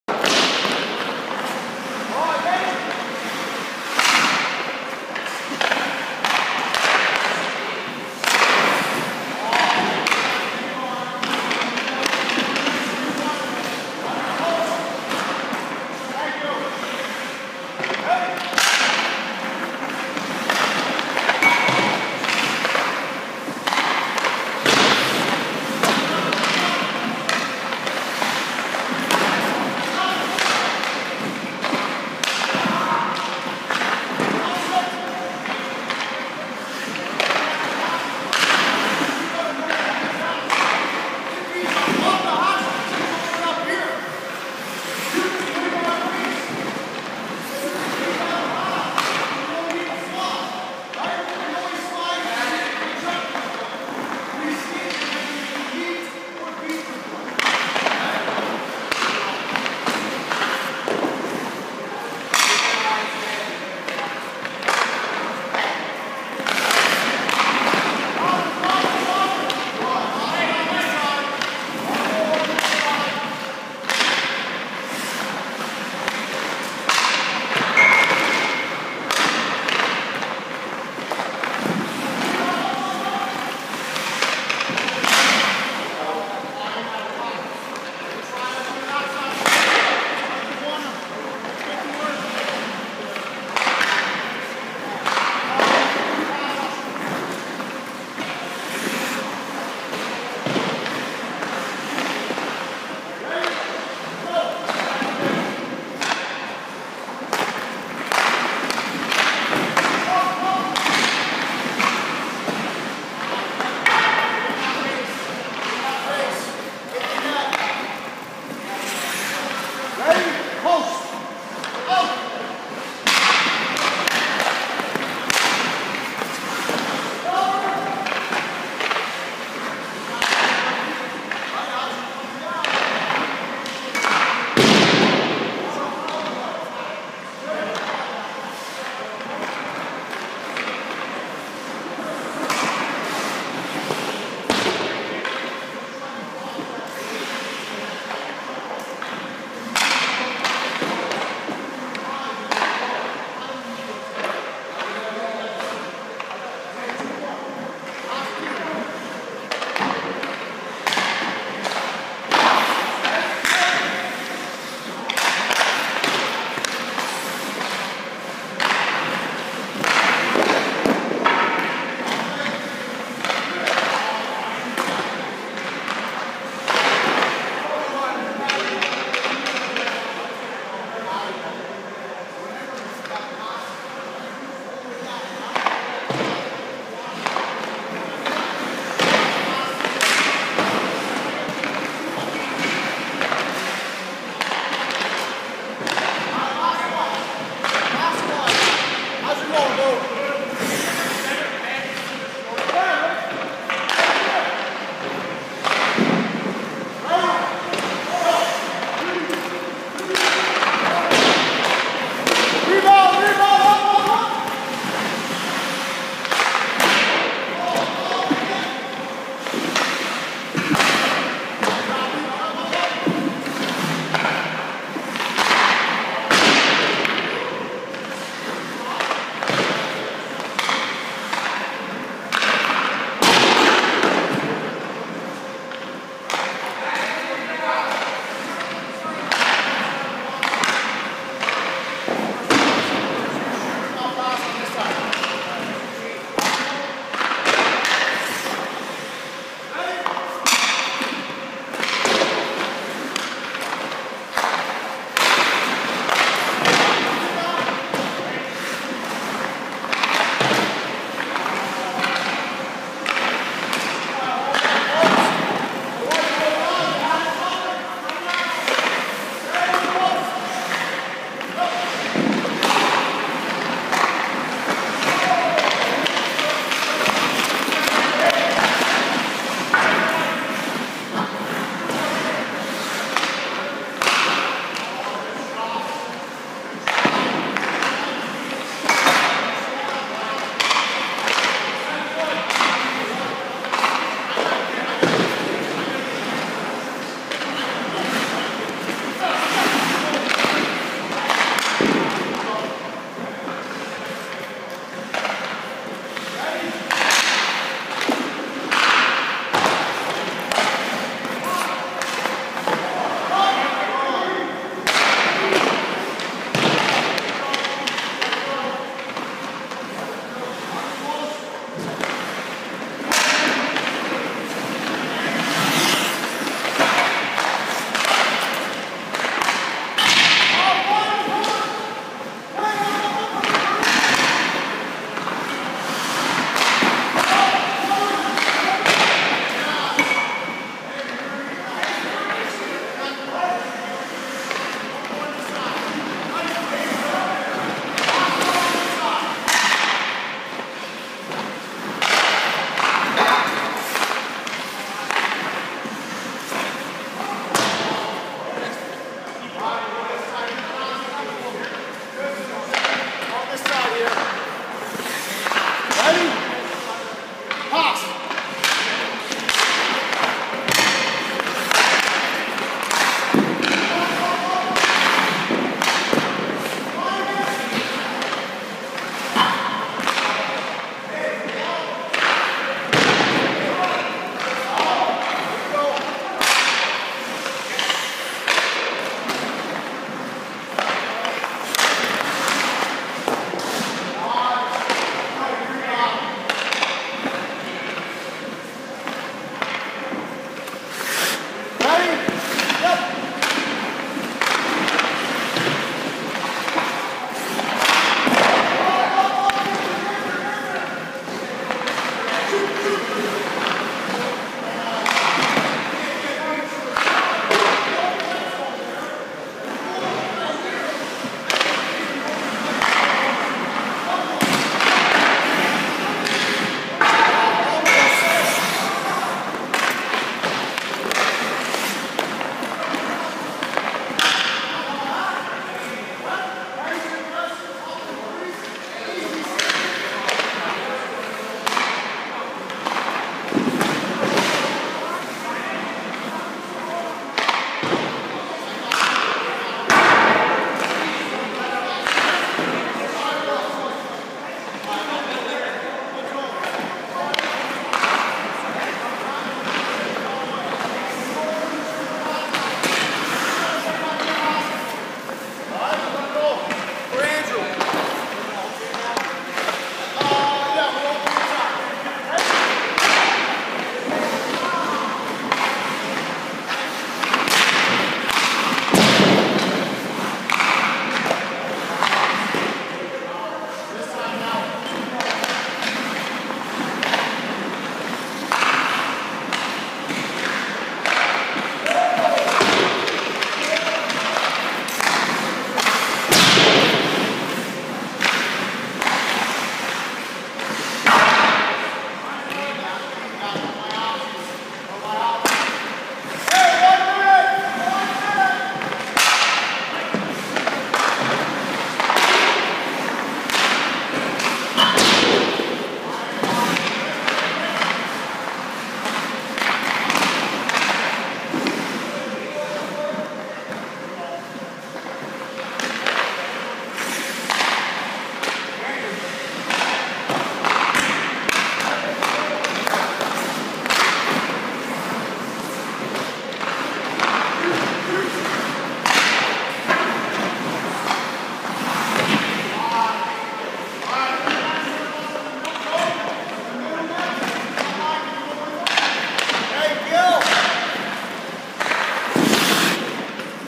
The Goalie Doctor - Joe Lonergan, Tristan Lowe, Andrew Craig - 6/8/21
Coach Matt Tendler - PNY Sports Arena - Goalie Clinic